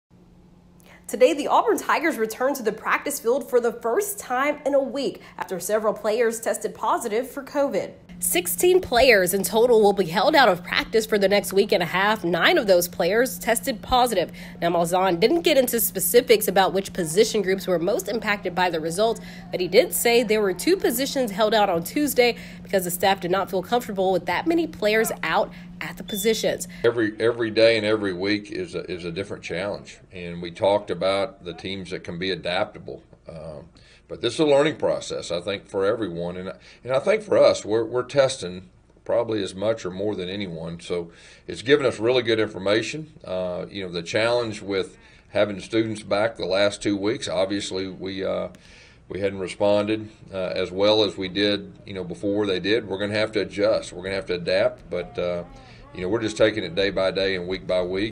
Auburn Football returns to practice for first time in a week
The Auburn Tigers returned to the practice field after a missing a week of practice mainly due to COVID reasons.